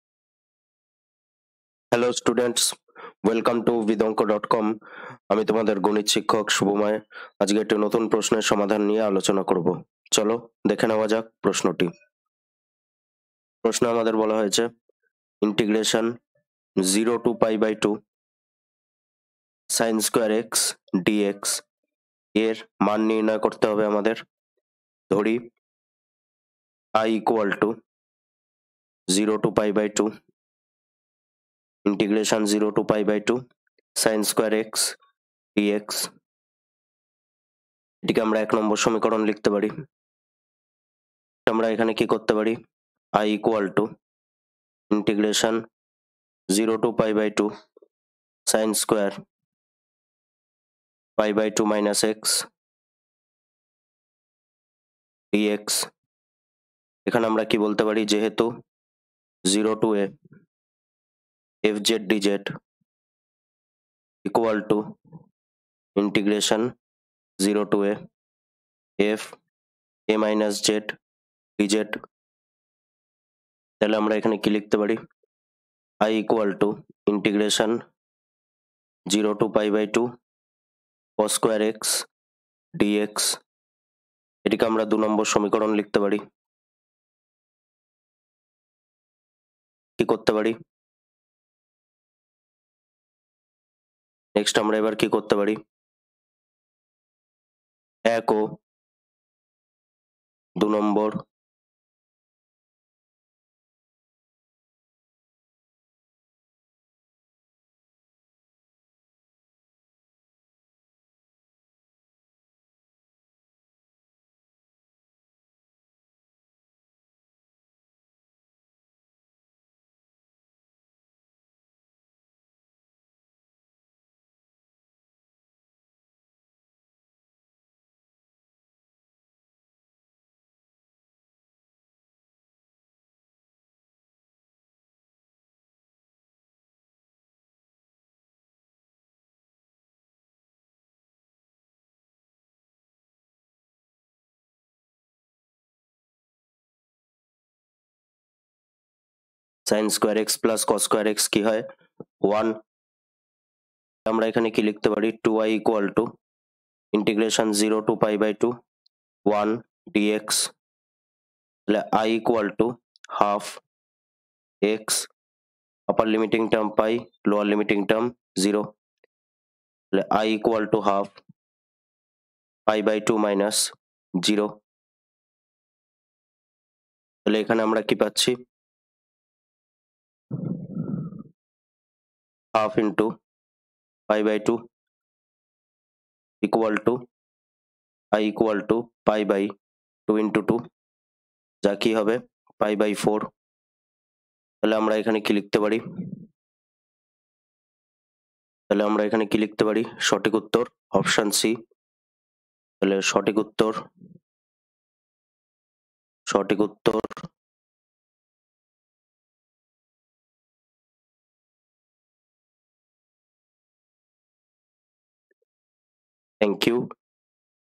WB Board Class 12 Mathematics Book Solution in Bengali - S N Dey Exercise Question: 16A.1.13
Get free Maths book video and PDF solutions in Bengali for S N Dey. Free answer and explanation through video for question 16A.1.13, of Chapter Definite Integral from S N Dey followed for Class 12, West Bengal Council of Higher Secondary Education (WBCHSE).